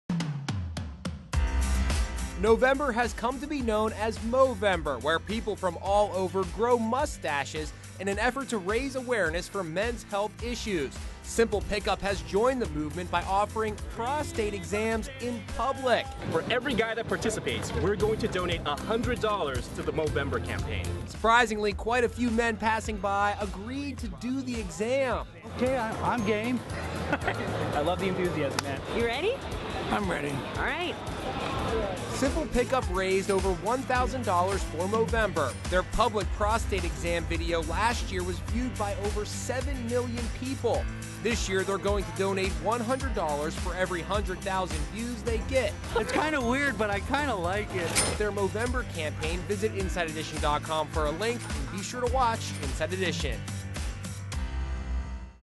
Men Agree to Free Prostate Exams In Public To Raise Money for Movember
The team at Simple Pickup me has decided to raise awareness for prostate cancer in a unique way. They visited Venice Beach and offered to give men free prostate exams in public. Simple Pickup's founders, Kong and Jesse, promised to donate $100 do...